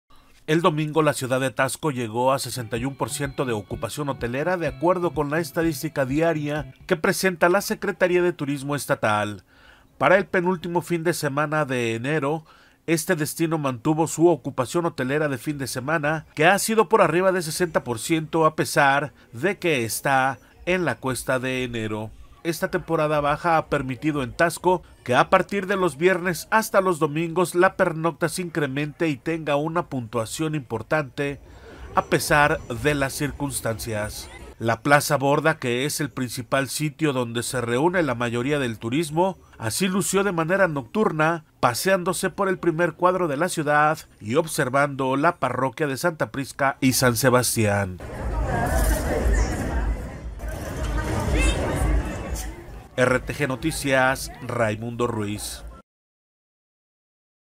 RTGnoticias | Taxco logra 61% de ocupación hotelera
El domingo, la ciudad de Taxco llegó a 61 por ciento de ocupación hotelera, de acuerdo con la estadística diaria que presentó la Secretaría de Turismo estatal.